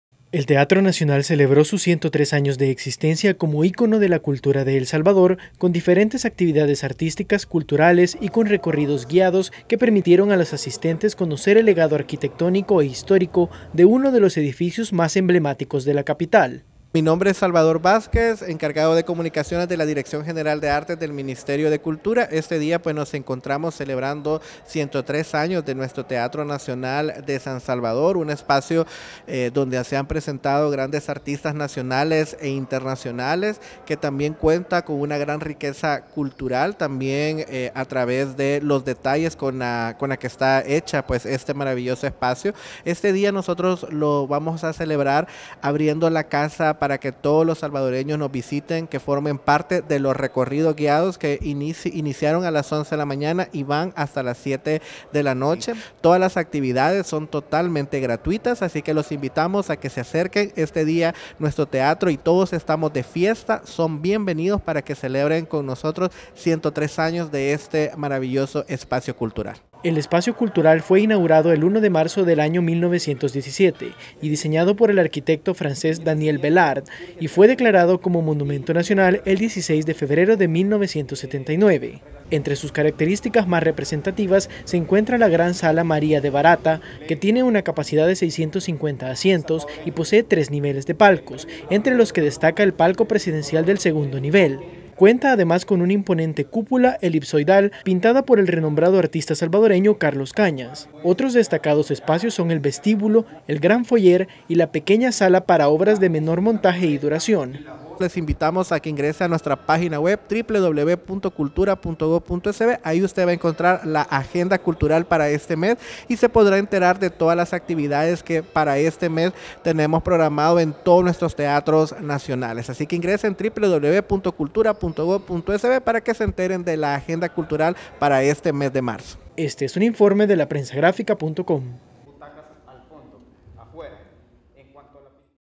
Teatro Nacional celebra sus 103 años como ícono cultural de El Salvador
El Teatro Nacional celebró sus 103 años de existencia como ícono cultural de El Salvador con diferentes actividades artísticas, culturales y con recorridos guiados que permitieron a los asistentes conocer el legado arquitectónico e histórico de uno de los edificios más emblemáticos de la capital.

¡Visítenos!